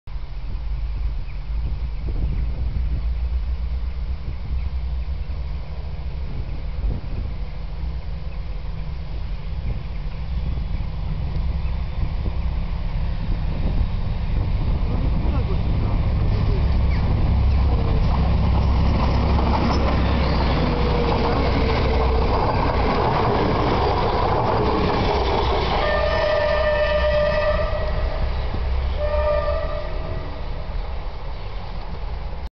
SU45-054
Z osobowym do Olsztyna wyjeżdża z Ełku